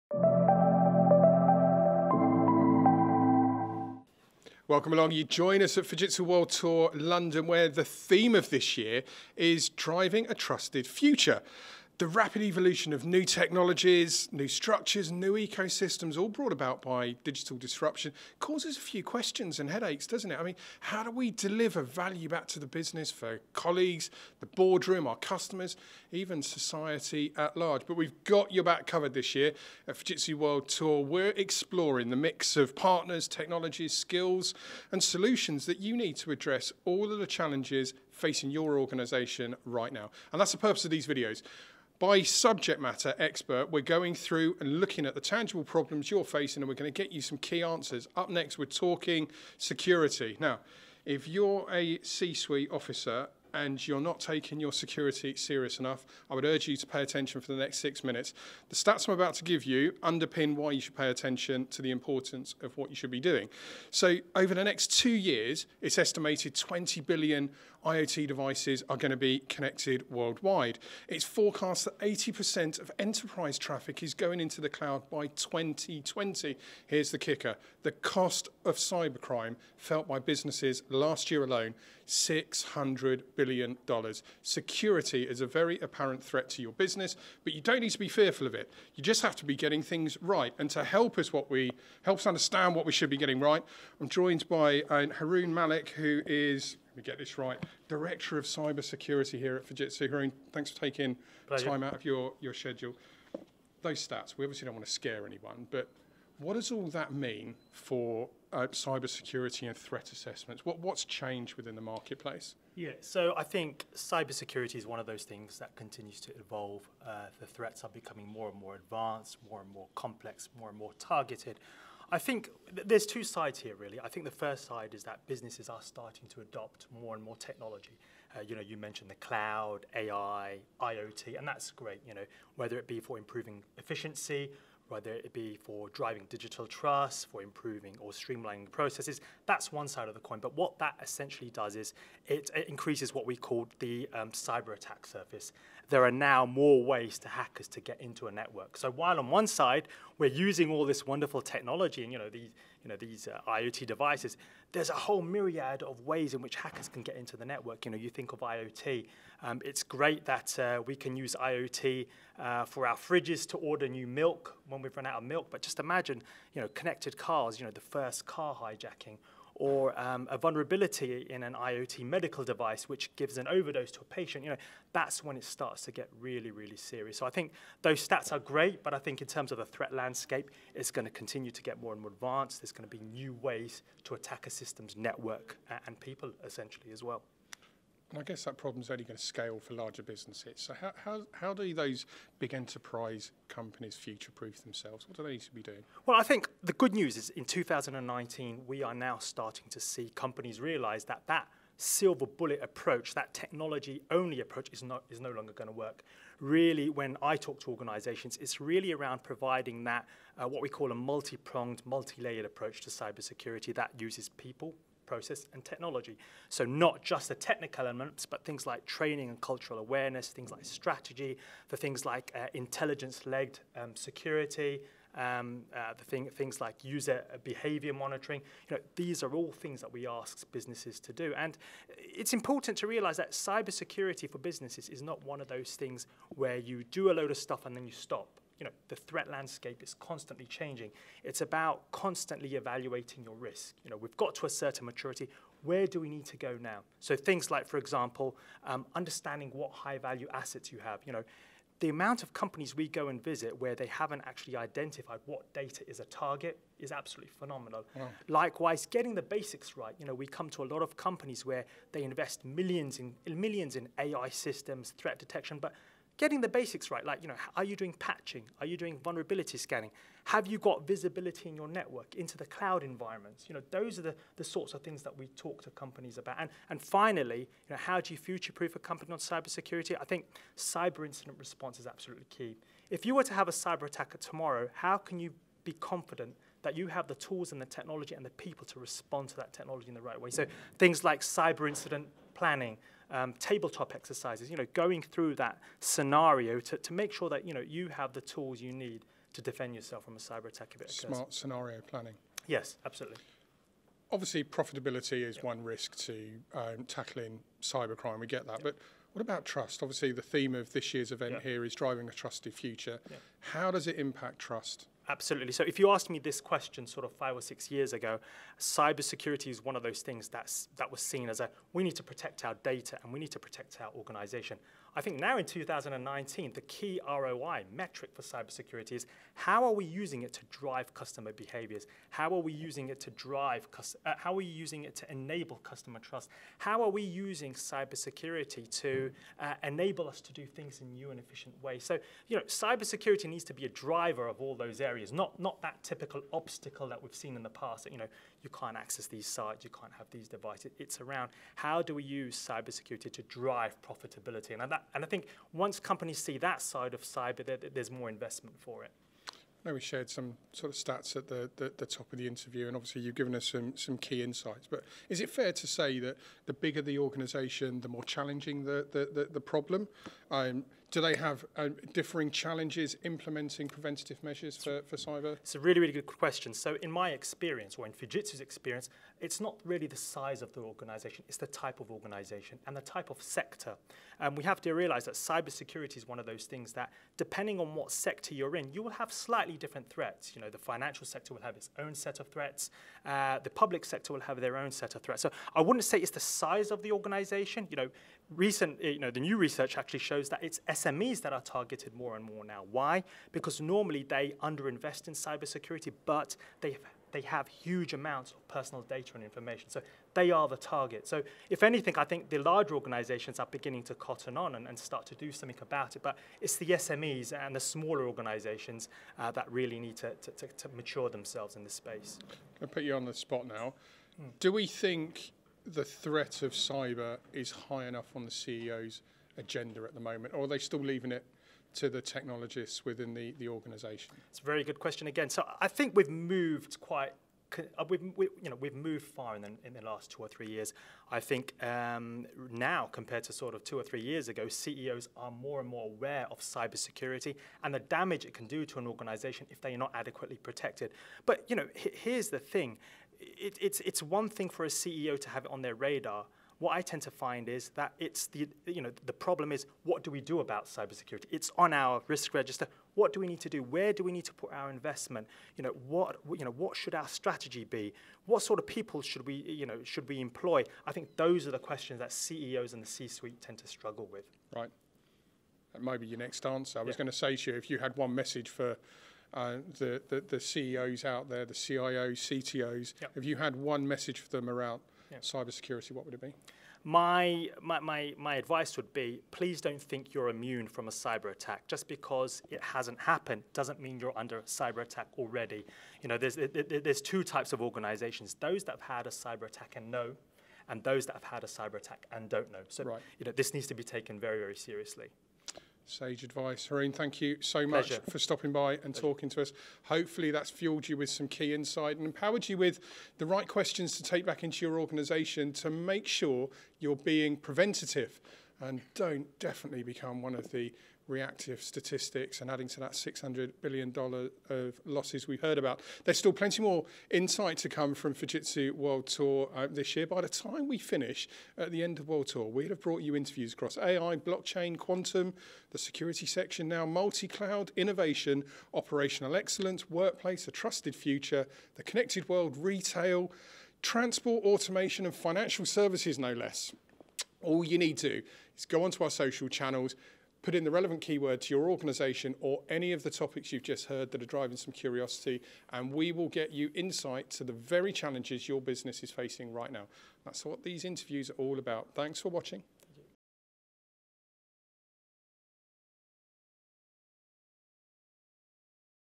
Enterprise and Cyber Security
Strong cybersecurity is critical for trust. But, does new tech mean new threats? Our Director of Cyber Security and Consulting, Haroon Malik, shares more at Fujitsu World Tour